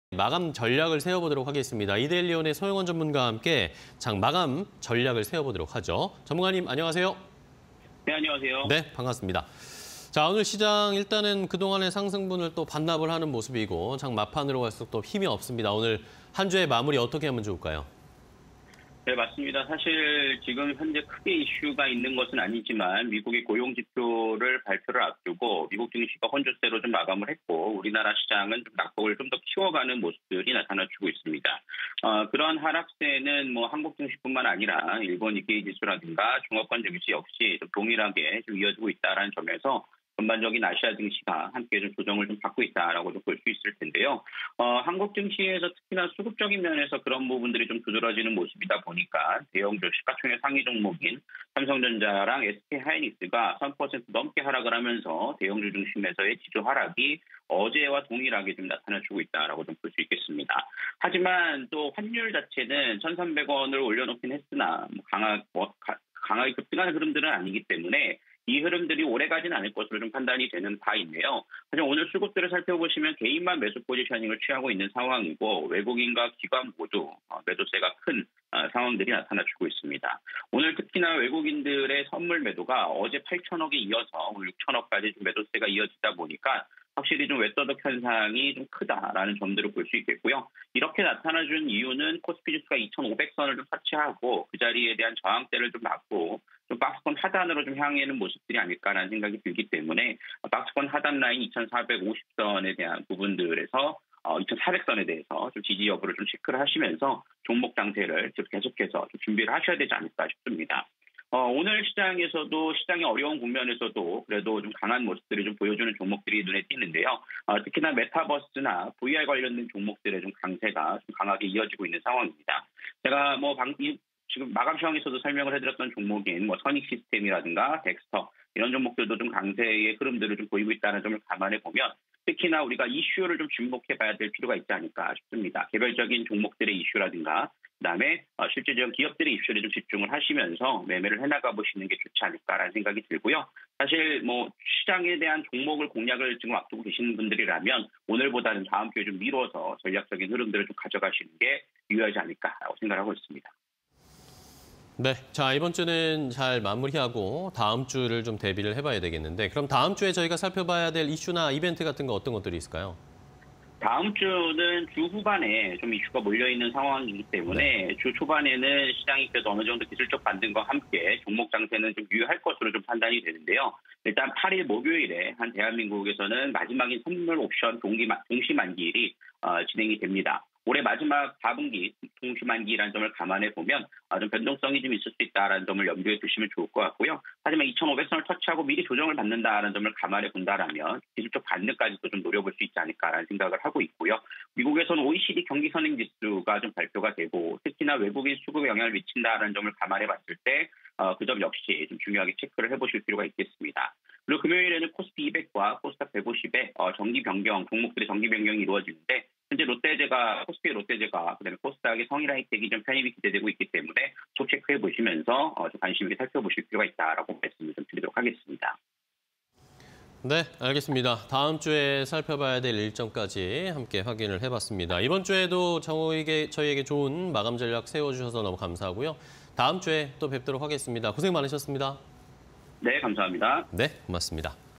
美 고용지표 발표 앞두고 시장 혼조세_서용원 마감 전략 (20221202)
- 美 고용지표 발표 앞두고 시장 혼조세
- 외국인 연일 매도세… 지수 조정

Q. 다음 주, 증시 일정 및 투자 전략은?
- 8일 선물옵션 동시만기일
- 8일 OECD 경기선행지수 발표
- 9일 코스피200ㆍ코스닥150 정기변경
- 9일 롯데제과ㆍ성일하이텍 편입 기대